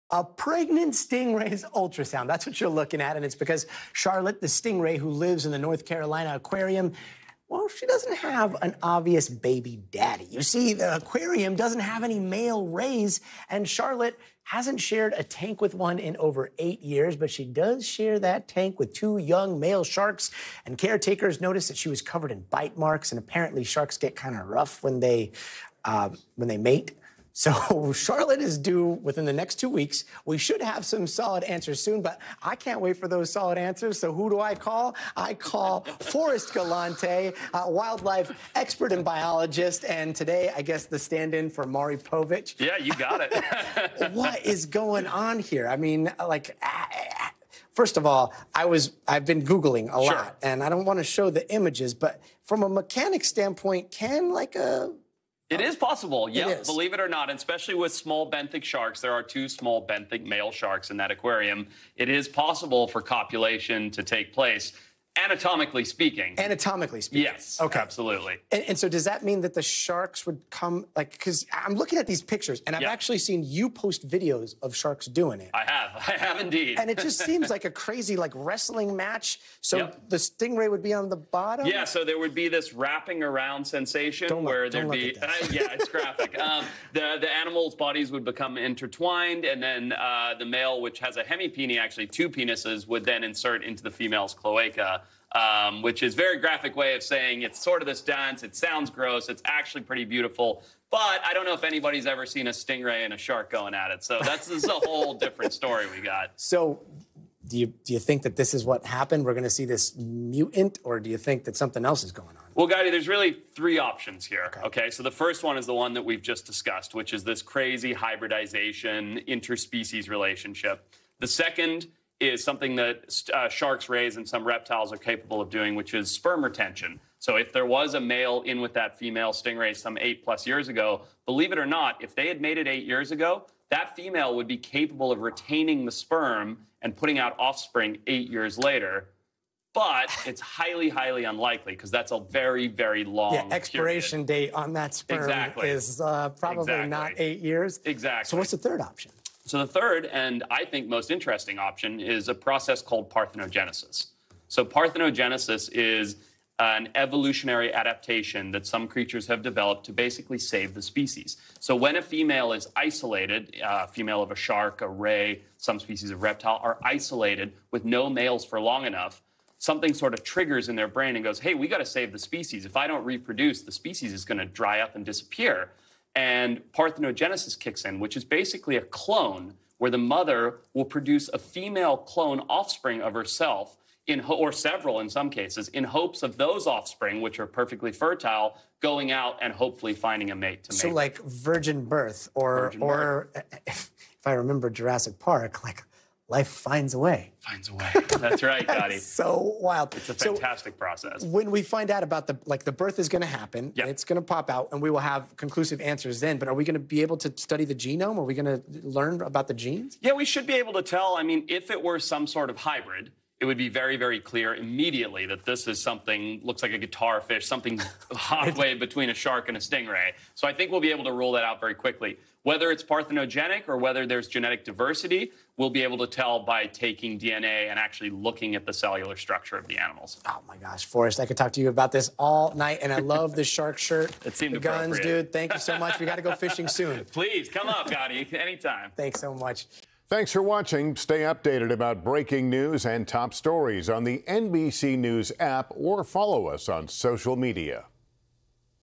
Mysterious stingray pregnancy leaves many to wonder if shark is father
Charlotte the stingray has left many to wonder after becoming pregnant, as there are no male rays in her North Carolina aquarium. Wildlife biologist Forrest Gallante explains the mystery behind this pregnancy and how a shark could be the culprit.